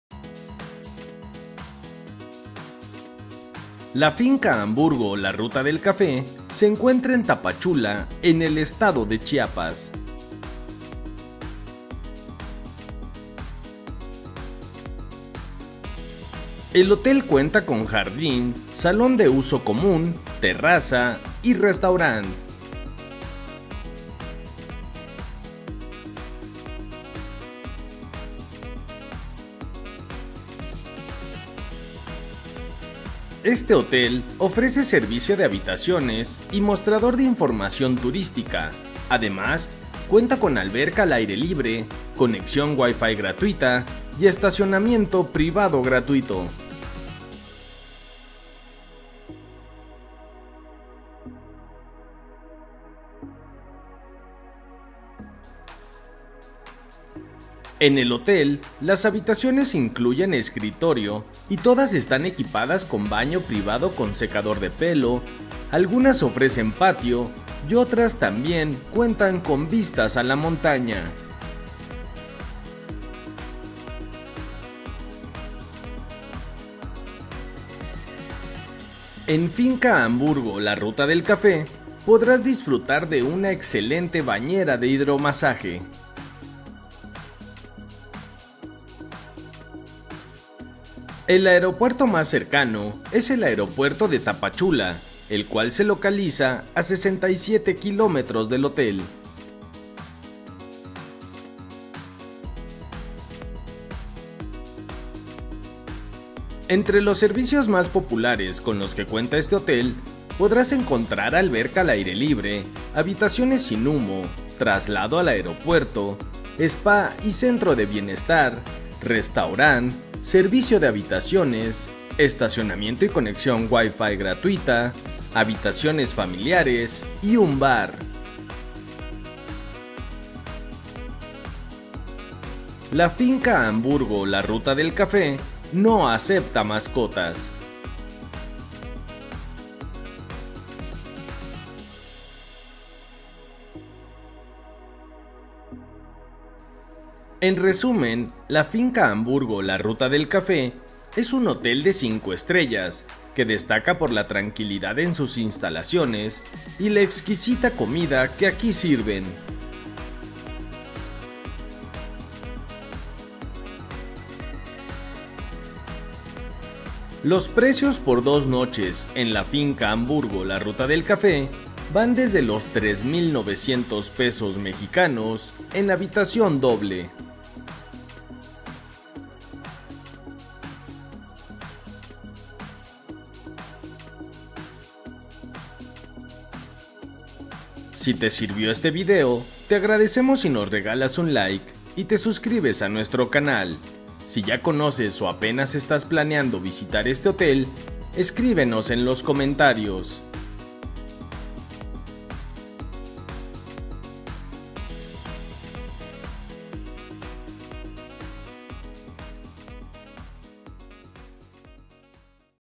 Finca Hamburgo La Ruta del Café en Tapachula
Finca Hamburgo La Ruta del Café.

La Finca Hamburgo La Ruta del Café es un hotel de 5 estrellas que te recomendamos visitar en tu próximo viaje a Tapachula, en el estado de Chiapas.

Créditos:
Video editado en su totalidad por este canal "Turimexico.com"
Voz: Rubén Zepeda Carreiro

¡SÍGUENOS!